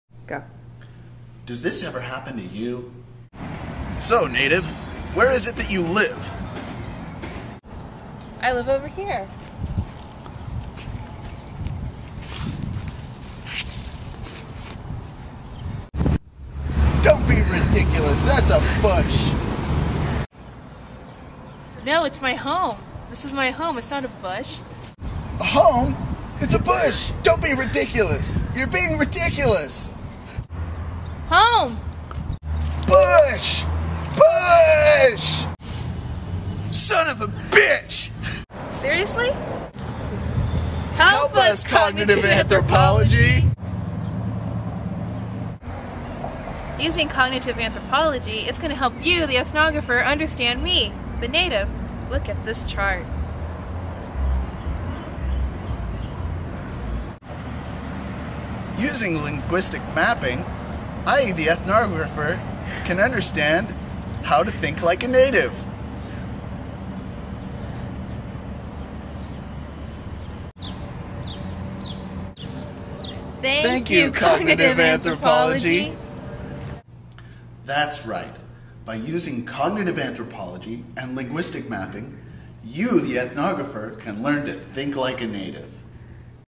A Cognitive Anthropological Approach to Ethnography
In a world of Direct Democracy, a team of Cognitive Anthropologists are vying to recieve grant monies to do anthropologically study the People of the planet Skandalos, known as the Skandalosians